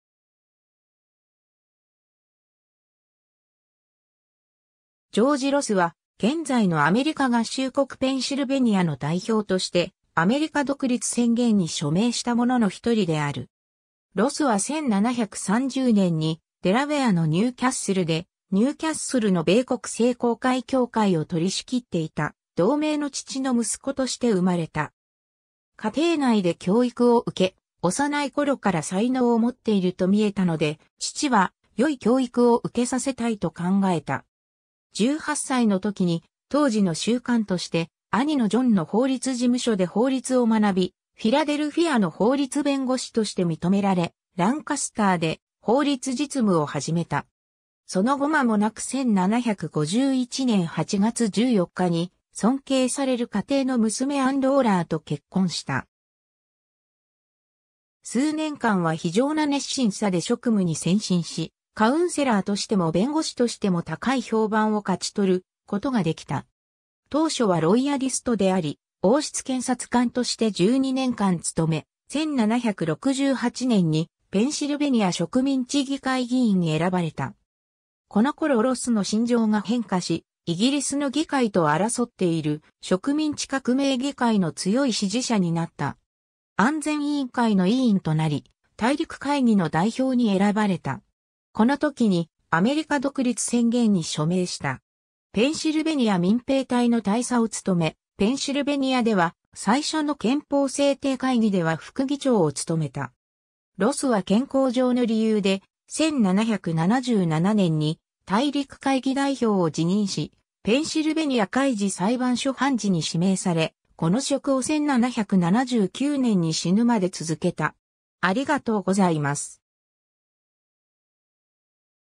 ジョージ・ロス
ジョージ・ロス（英: George Ross、1730年5月10日-1779年7月14日）は、現在のアメリカ合衆国ペンシルベニアの代表としてアメリカ独立宣言に署名した者の一人である。
 ロスは1730年にデラウェアのニューキャッスルで、ニューキャッスルの米国聖公会教会を取り仕切っていた同名の父の息子として生まれた。
家庭内で教育を受け、幼い頃から才能を持っていると見えたので、父は良い教育を受けさせたいと考えた。
18歳の時に当時の習慣として兄のジョンの法律事務所で法律を学び、フィラデルフィアの法律弁護士として認められ、ランカスターで法律実務を始めた。
その後間もなく1751年8月14日に尊敬される家庭の娘アン・ローラーと結婚した。
数年間は非常な熱心さで職務に専心し、カウンセラーとしても弁護士としても高い評判を勝ち取ることができた。
 当初はロイヤリストであり、王室検察官として12年間務め、1768年にペンシルベニア植民地議会議員に選ばれた。
このころロスの心情が変化し、イギリスの議会と争っている植民地革命議会の強い支持者になった。
 安全委員会の委員となり、大陸会議の代表に選ばれた。
このときにアメリカ独立宣言に署名した。
ペンシルベニア民兵隊の大佐を務め (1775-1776)、ペンシルベニアでは最初の憲法制定会議では副議長を務めた。
ロスは健康上の理由で、1777年に大陸会議代表を辞任し、ペンシルベニア海事裁判所判事に指名され、この職を1779年に死ぬまで続けた。